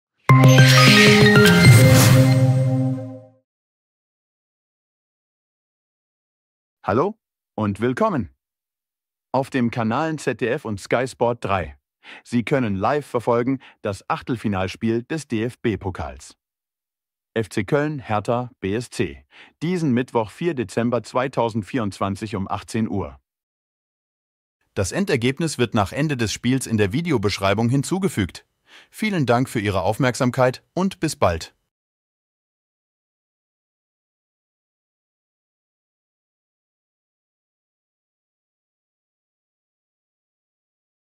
DFB Pokal : Köln 2 - 1 Hertha live auf ZDF | Sky Sport 3 | 04.12.2024 um 18 Uhr
Auf dem kanalen ZDF  und Sky Sport 3 , Sie können live verfolgen das Achtelfinalspiel des DFB-Pokals : FC Köln - Hertha BSC  , diesen Mittwoch 4 Dezember 2024 um 18 Uhr.

Spielergebnis : Köln 2 - 1 Hertha